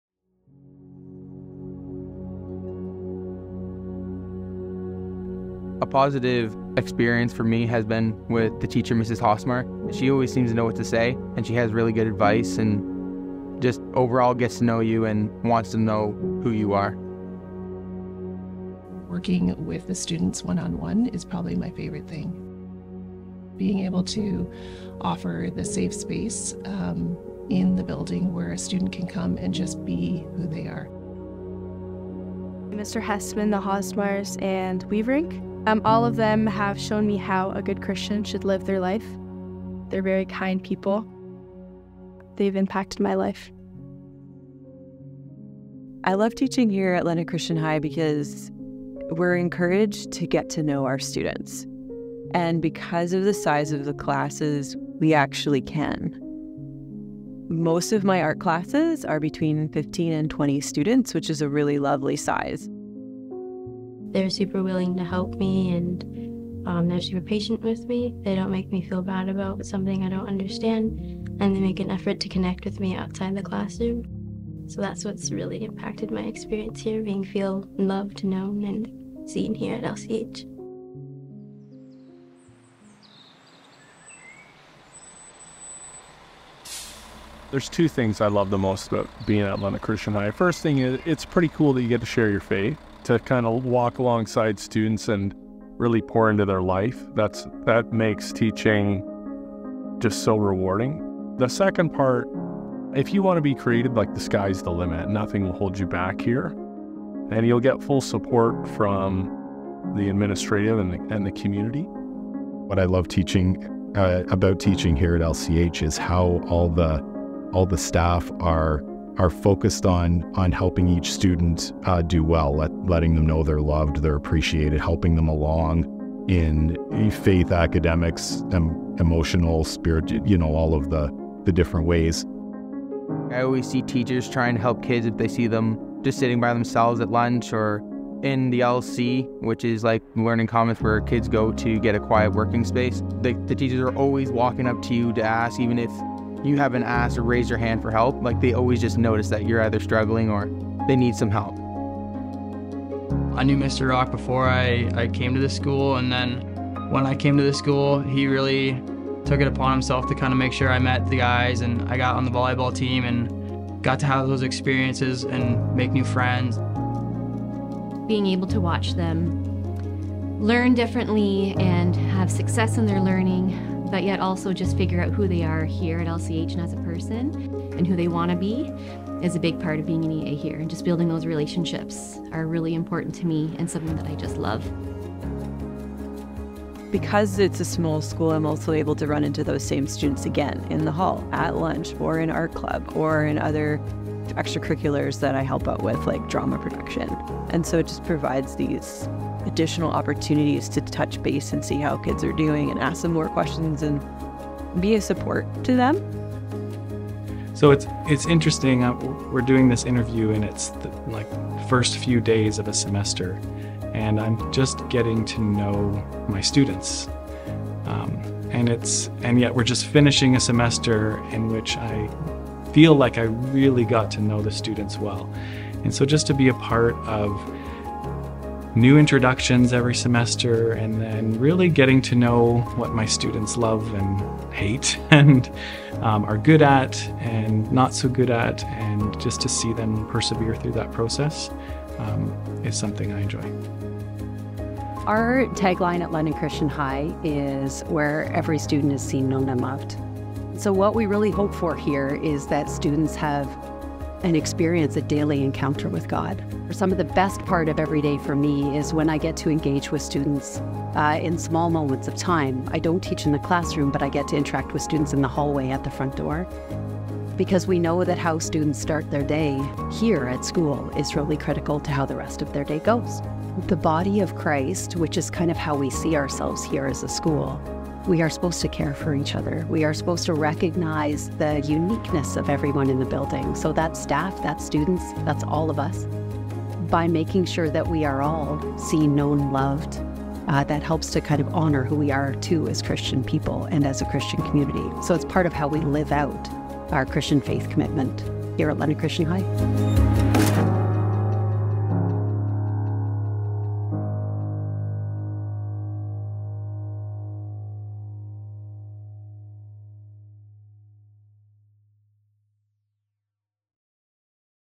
The Sky's The Limit at London Christian High
The sky's the limit for teachers and students at London Christian High. Transform your high school experience with us! It's not too late to make a change.